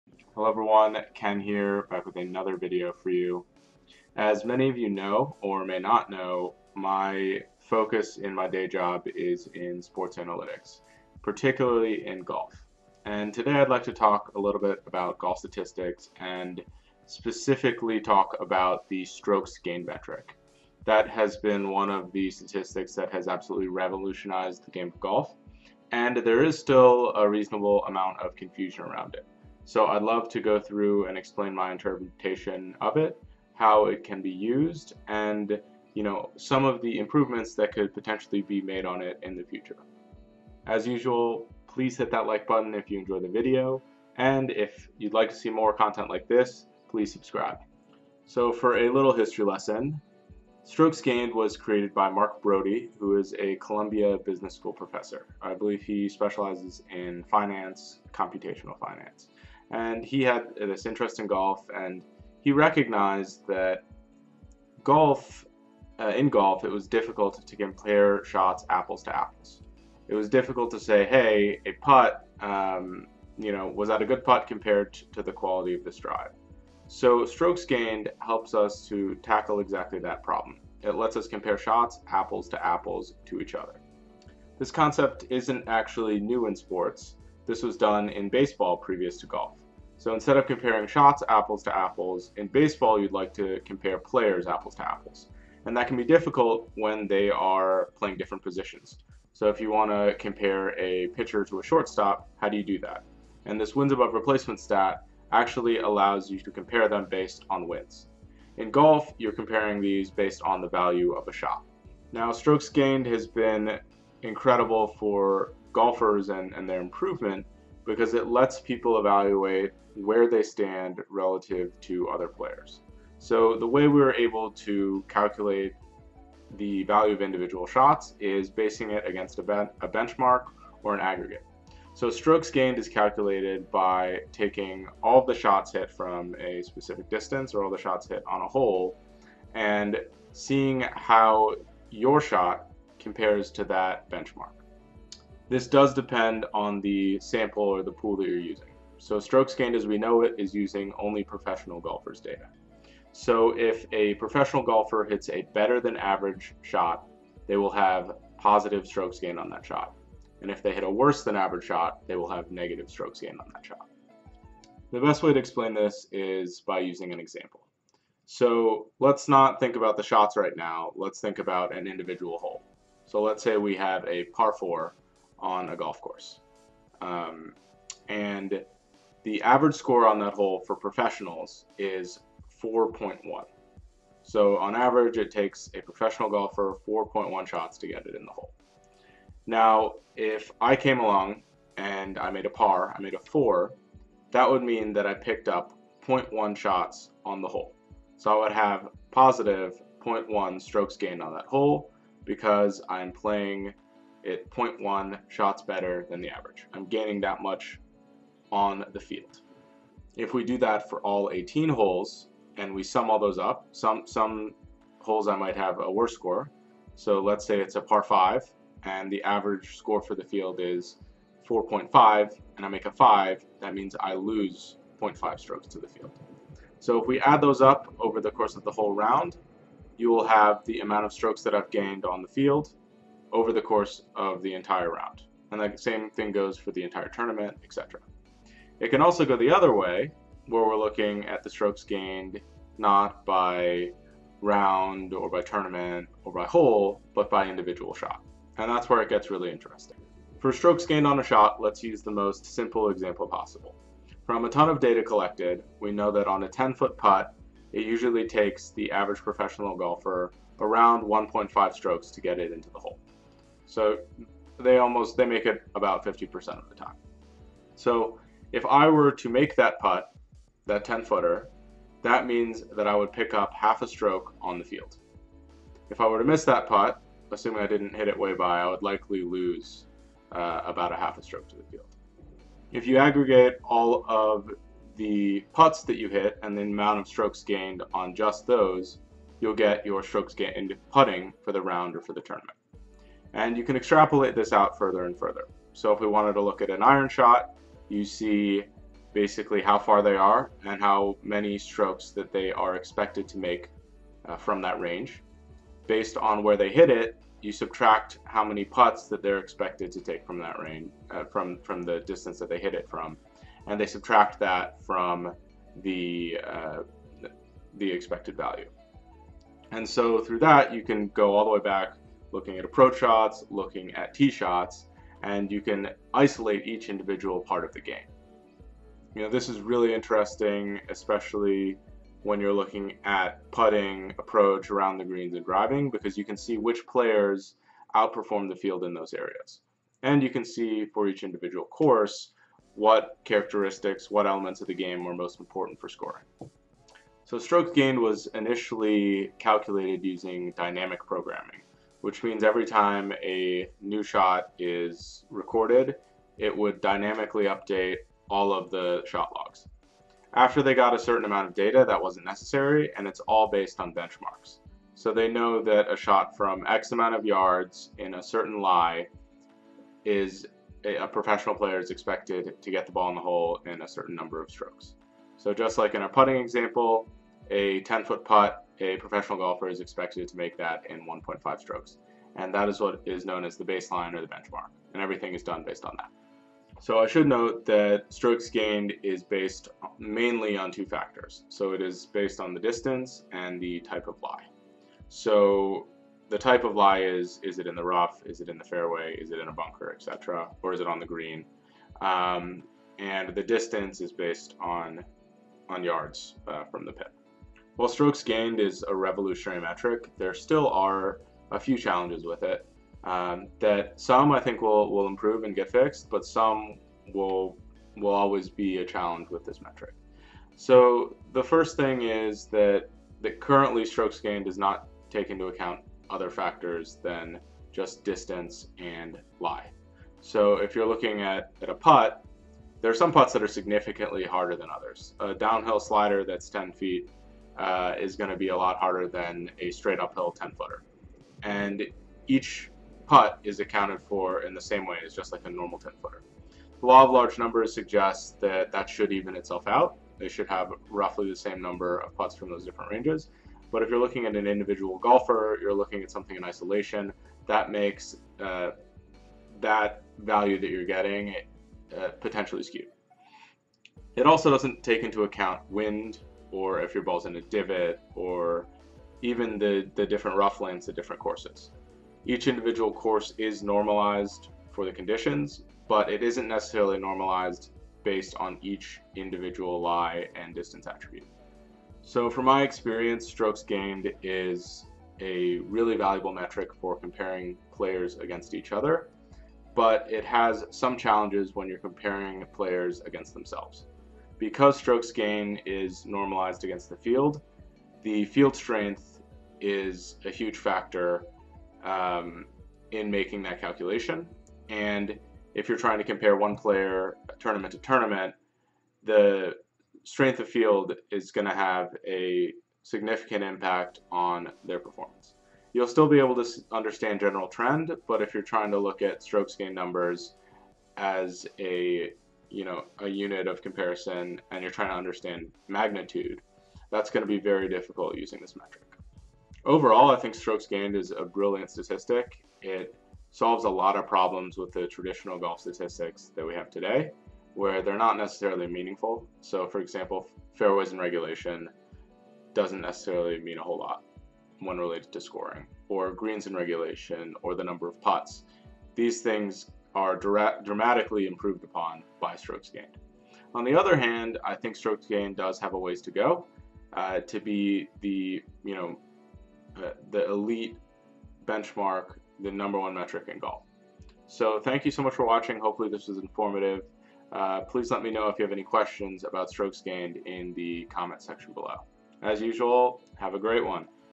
Golf STATS: Strokes Gained Explained
In this video I talk about the statistic that changed golf: Strokes Gained. Strokes gained was created my Mark Broadie a Colombia business school professor.

Strokes gained is a normalized metric that allows us to compare type of shot apples to apples. They are compared using fractions of strokes. This is very similar to how wins above replacement is used in baseball.

Strokes gained is also an aggregated / normalized metric. In order to get strokes gained, we compare each shot, hole, round, and tournament to the average. If a player shoots 72 and the average score for the day is 73, then they have +1 strokes gained on the round. Players who play better than average should always have positive total strokes gained and players who perform worse than average should always have negative strokes gained total.